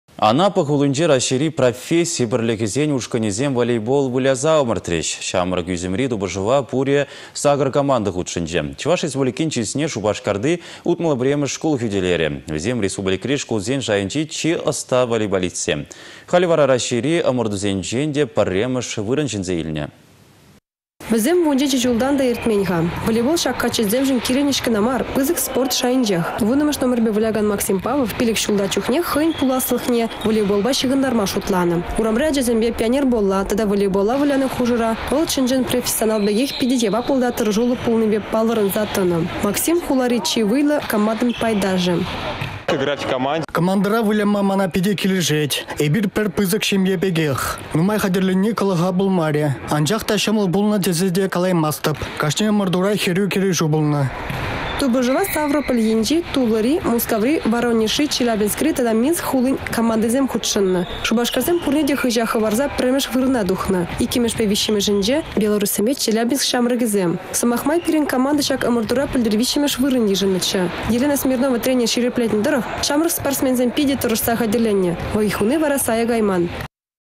Çĕнтерÿсемпе савăнтараççĕ
Анапа хулинче Раççейри професси пĕрлĕхĕсен ушкăнĕсем волейбол выляса ăмăртрĕç. Çамрăк ÿсĕмри тупăшăва пурĕ сакăр команда хутшăнчĕ. Чăваш Республикин чысне Шупашкарти 61-мĕш шкул хÿтĕлерĕ. Вĕсем республикăри шкулсен шайĕнчи чи ăста волейболистсем. Халĕ вара Раççейри ăмăртусенчен те пĕрремĕш вырăн çĕнсе илнĕ.